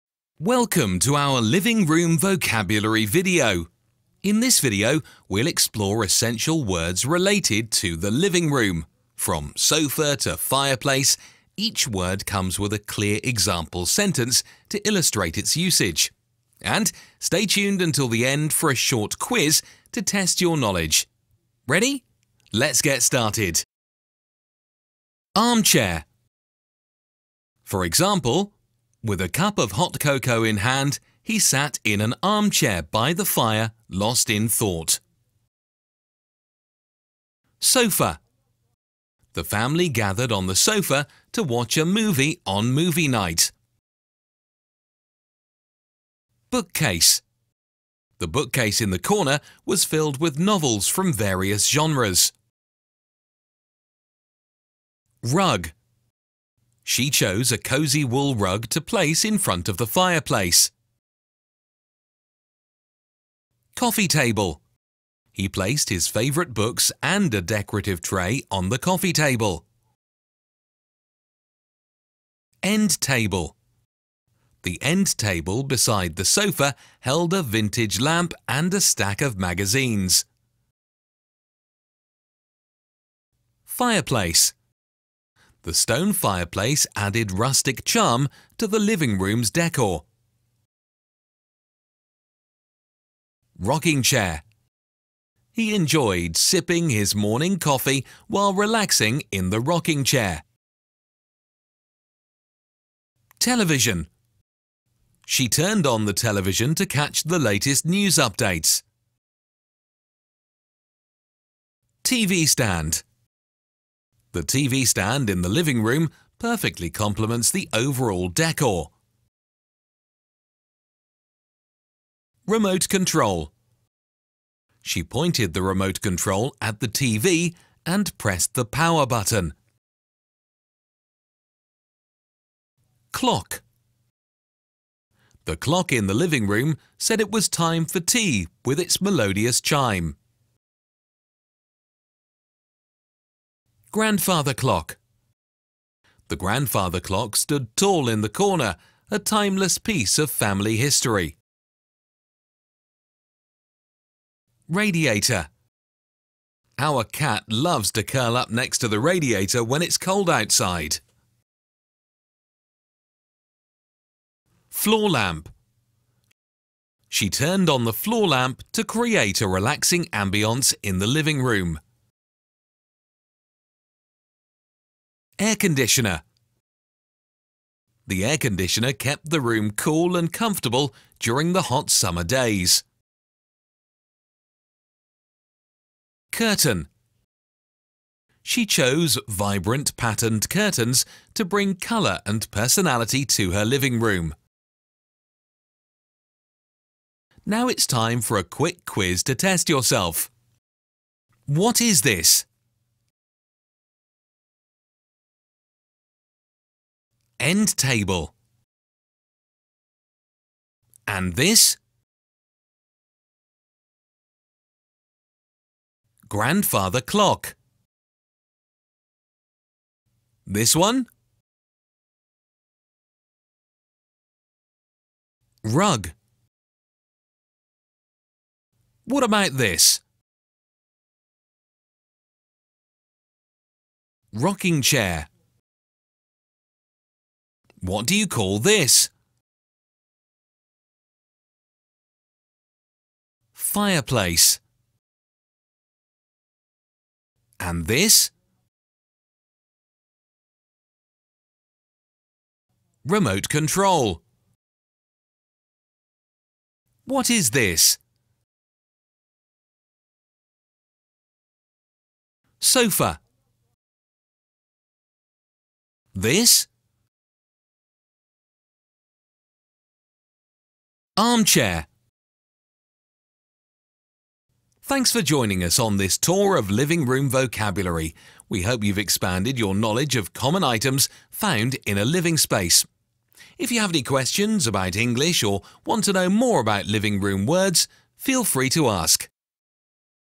Living Room Vocabulary
Welcome to our Living Room Vocabulary Video!
In this video, we'll explore essential words related to the living room. From sofa to fireplace, each word comes with a clear example sentence to illustrate its usage.